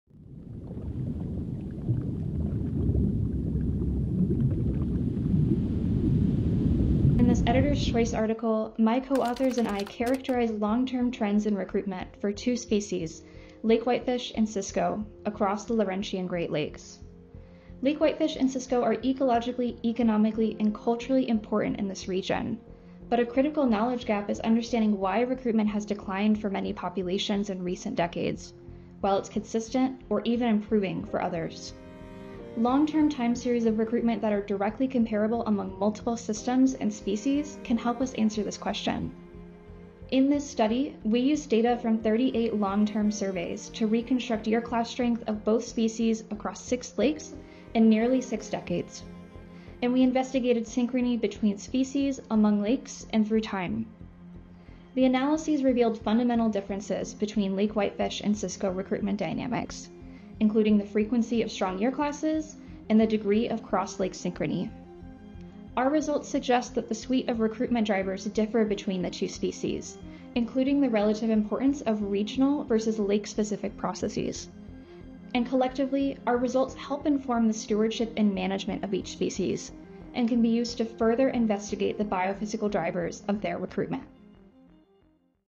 Editor’s Choice-Nearly 6 decades of recruitment trends for Lake whitefish & Cisco in the Great Lakes
How do fish recruitment dynamics shape Great Lakes fisheries?

The latest Editor’s Choice article in ICES Journal of Marine Science examines species-specific trends and drivers of variability across six decades.

📄 Reconstructing half a century of coregonine recruitment reveals species-specific dynamics and synchrony across the Laurentian Great Lakes